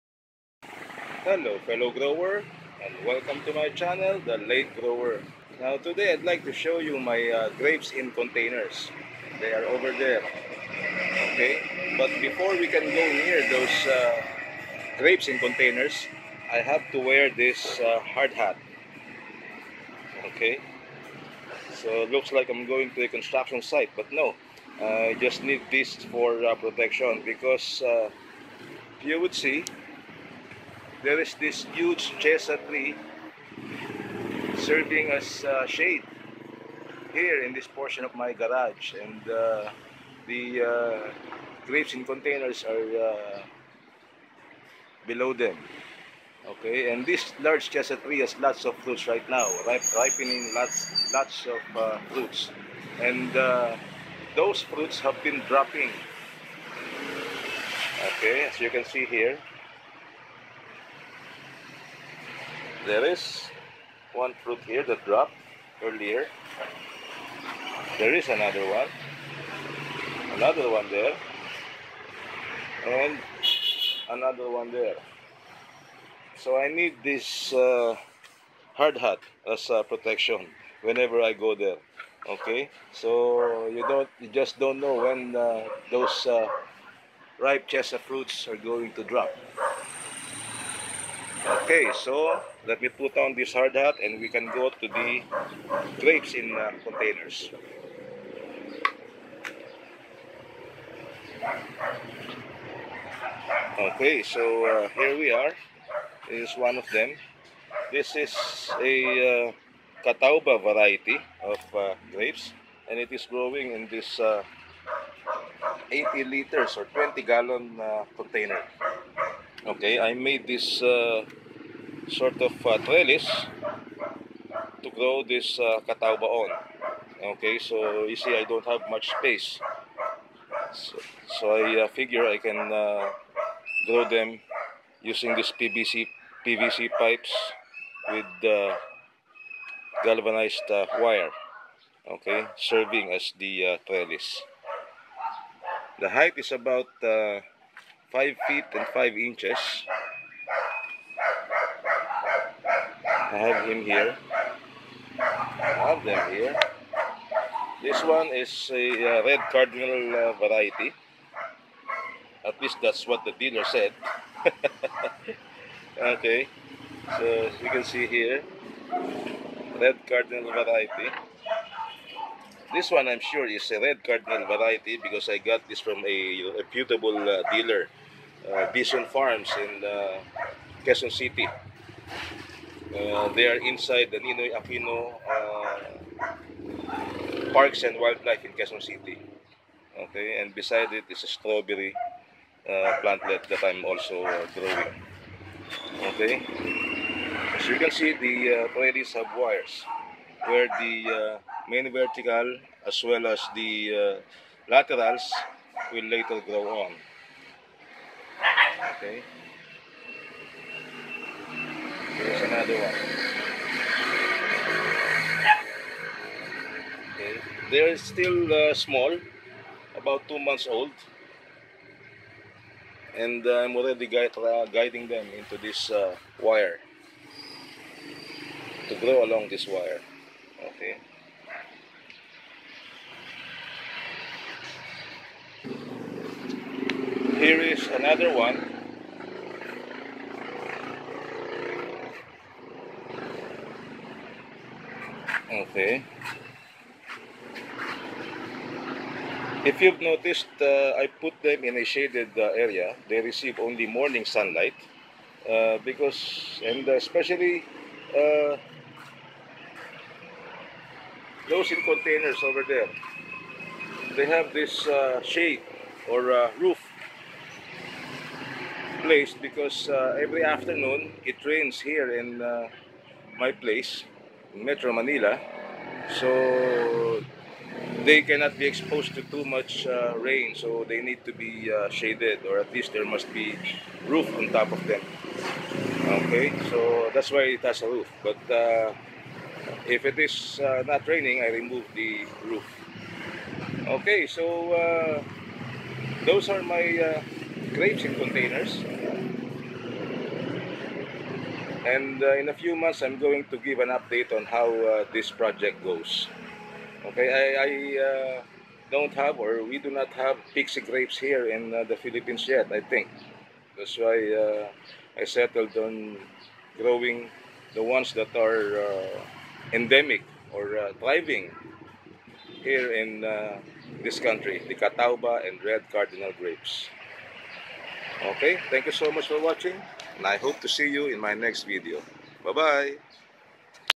Red Cardinal and Catawba Grapes in containers with built-in trellis
Due to lack of ample space and with the  desire to grow a number of grapevines in my small front yard I decided to grow grapevines in containers with built-in trellis.  Red Cardinal and Catawba are not dwarf varieties and they grow really fast but I will try to fit them in their designed trellis as best as I could applying the Umbrella kniffen  system.